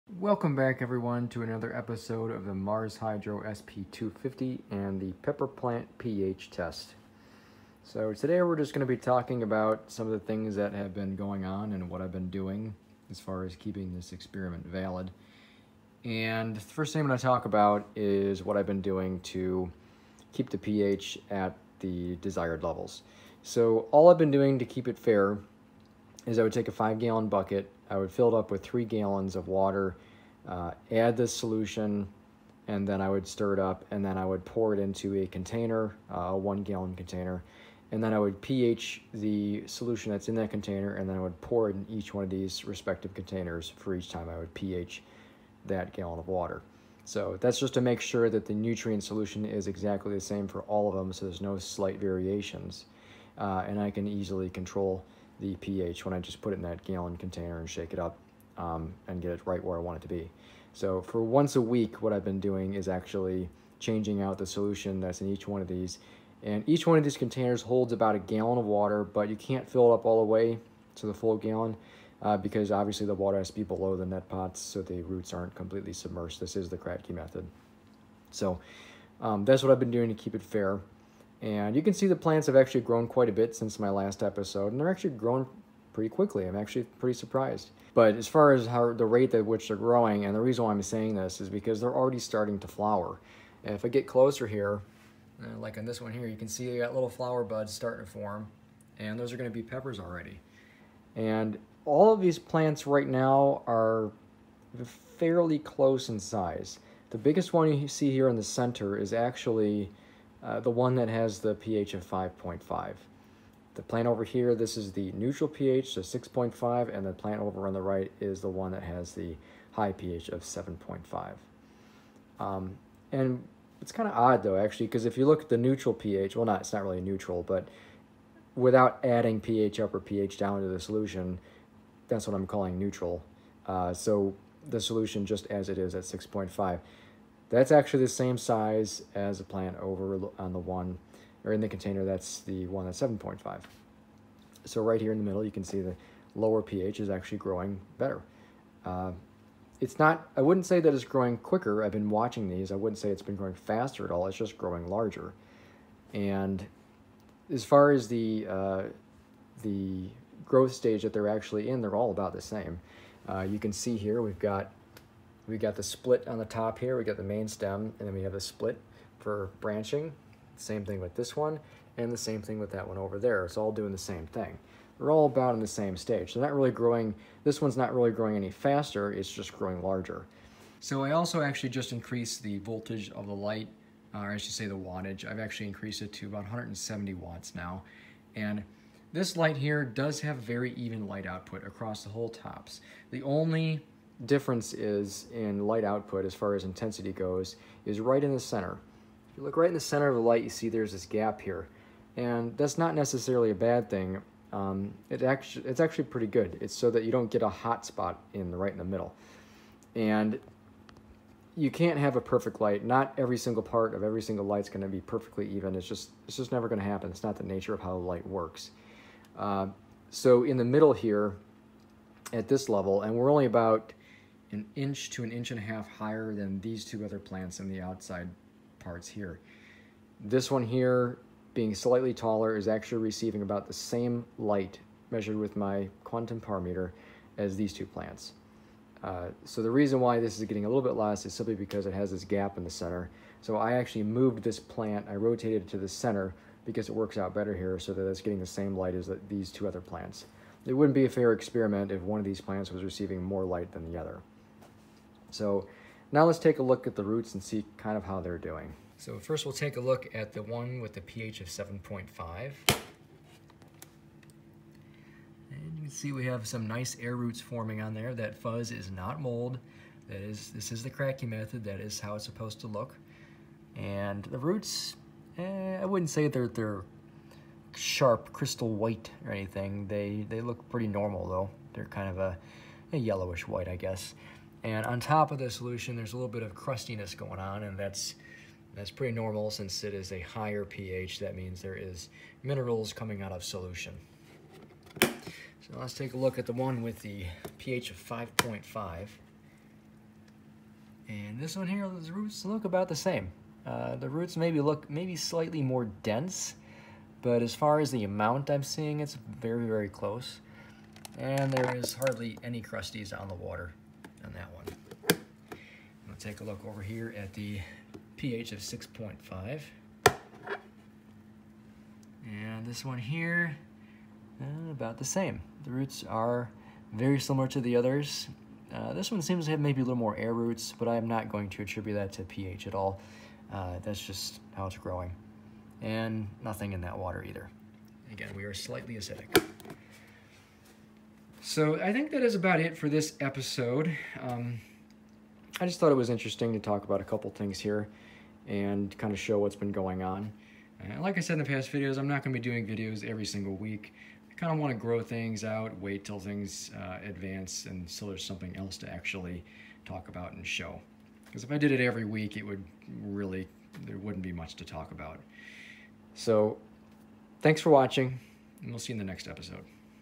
Growing Peppers at Different PH Levels | Mars Hydro SP 250 | Ep. 4
Coupon is no longer good for the TS or SP series.

In this series I take you along for the ride as I grow pepper plants in 3 containers hydroponically, at different PH levels. I am using the Kratky method to avoid PH fluctuations that would be introduced by aeration.